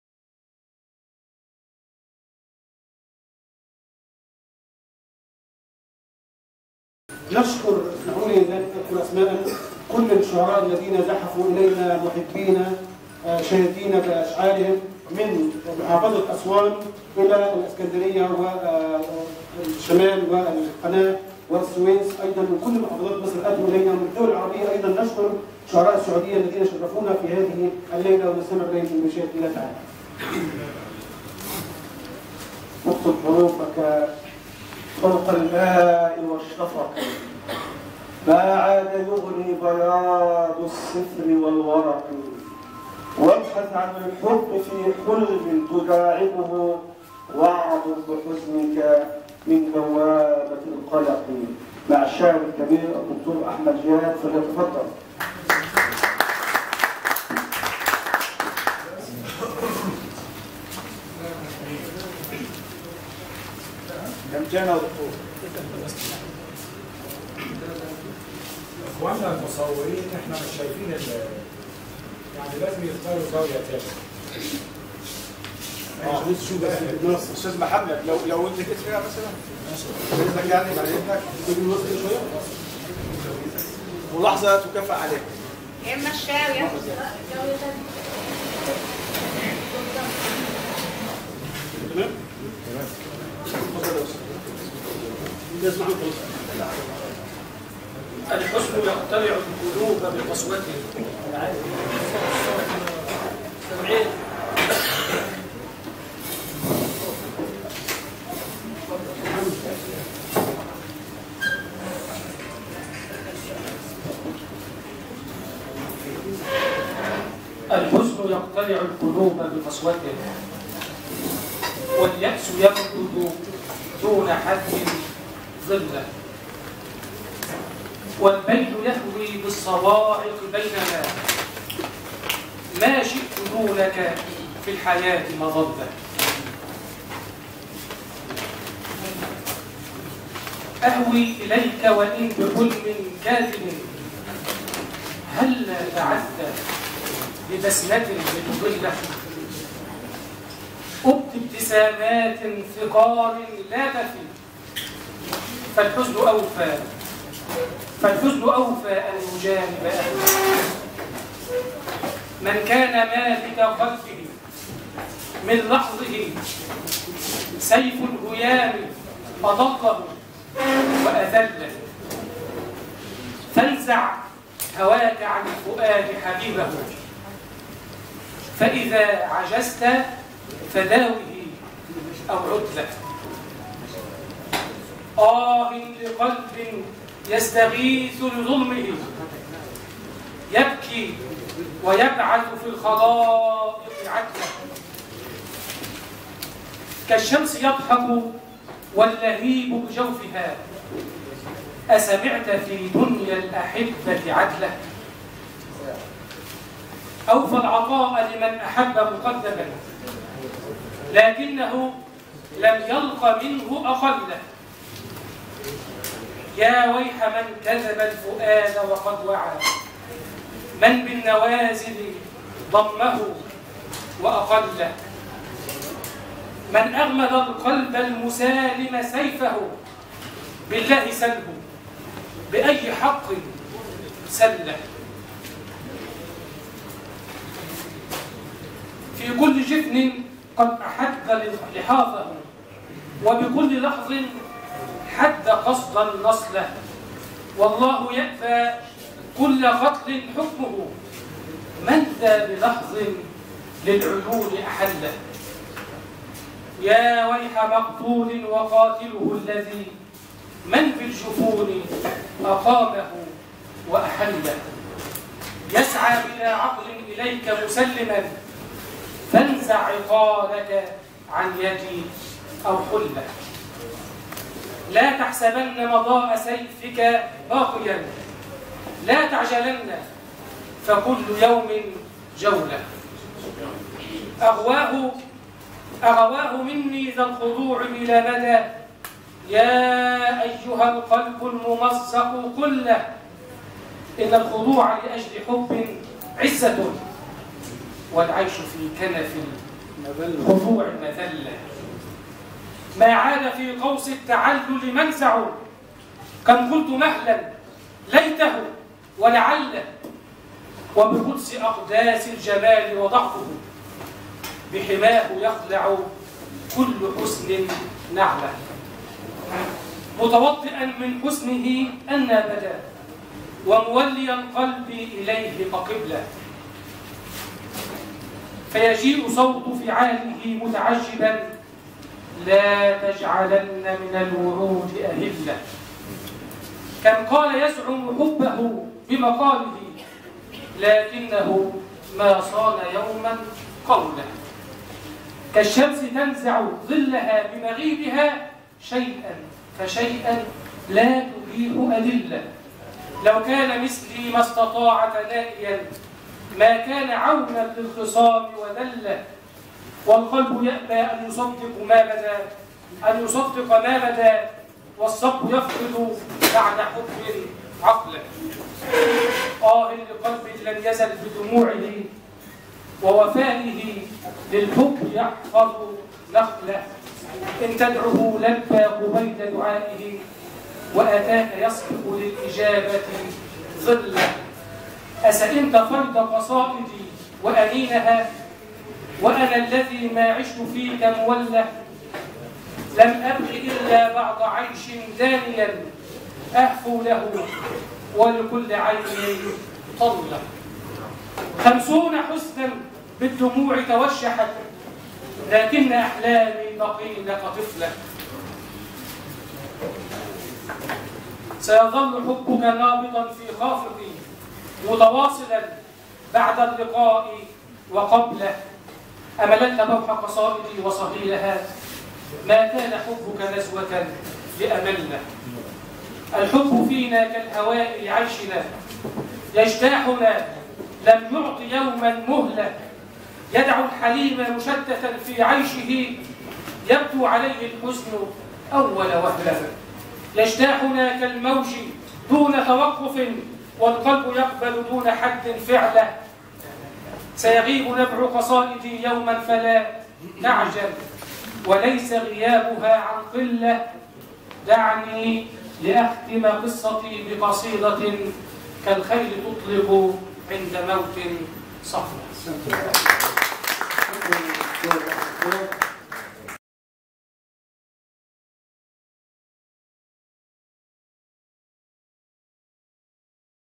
مهرجان بيت الشعراء العرب التاسع/الشاعر/ د /أحمد دياب/الأربعاء17/8/2022/نقابة المعلمين/حلمية الزيتون
أمسيةٌ شعريةٌ
مهرجان بيت الشعراء العرب التاسع
مؤسس ورئيس المهرجان الشاعر / خالد محمد قاسم
مدير الندوة والمنسق العام الشاعر / فضل محمد إبراهيم
الشاعر الدكتور / أحمد دياب
المكان : نقابة المعلمين الفرعية بشرق القاهرة
شارع البشري رقم 6 / أمام مدرسة البشري الابتدائية
محطة مترو حلمية الزيتون / القاهرة
الزمان : الأربعاء 2022/8/17